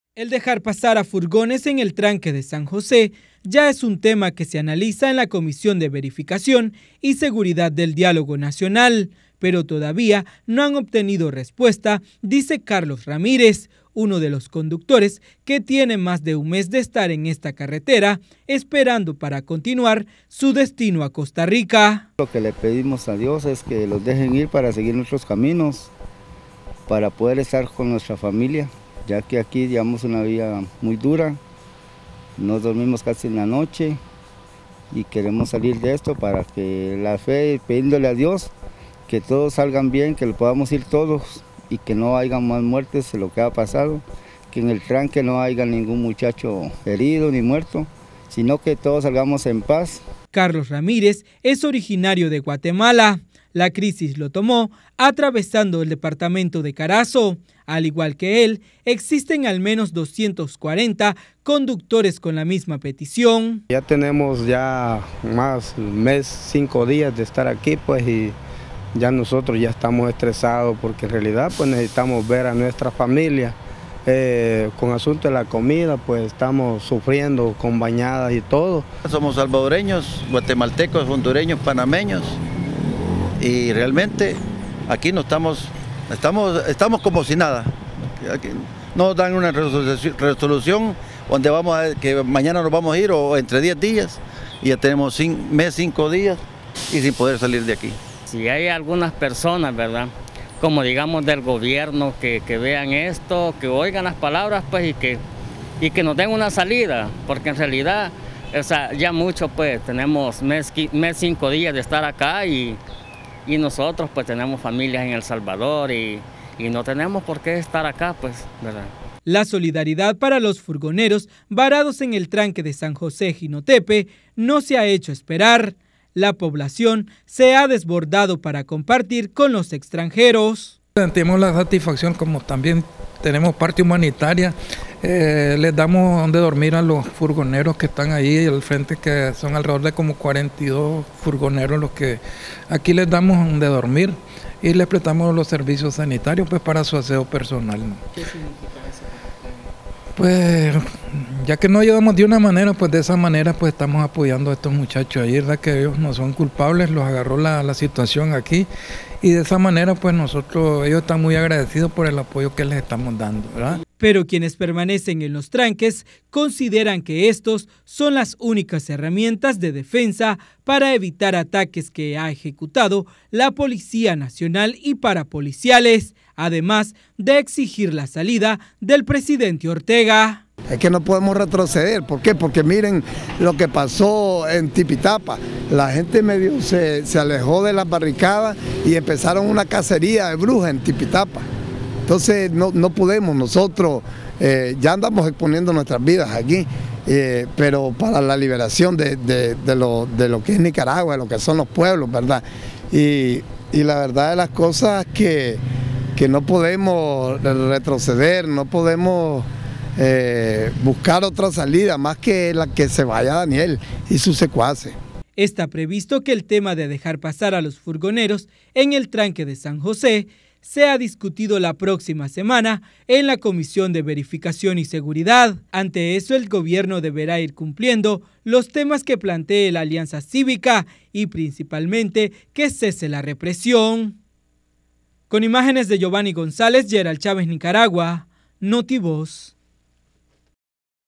Furgoneros varados en Jinotepe piden pasar
Desde hace un mes, cientos de furgones y camiones de carga pesada se encuentran varados en la carretera panamericana sur específicamente en Jinotepe Carazo, con un destino incierto.